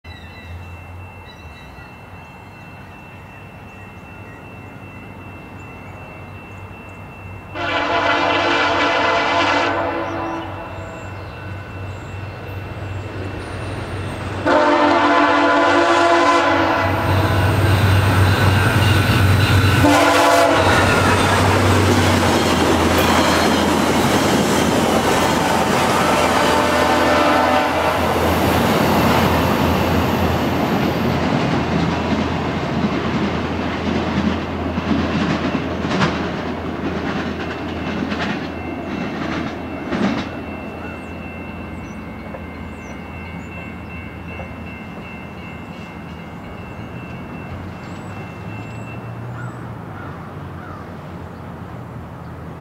VIA Train 61 Westbound October 18, 2023
Engines: 6416, 6413
Coaches:
Speed: 69 mph at detector 124
Location: Brockville VIA station, ON, Canada
Temperature: 8C
Origin: Montreal QC
Destination: Toronto ON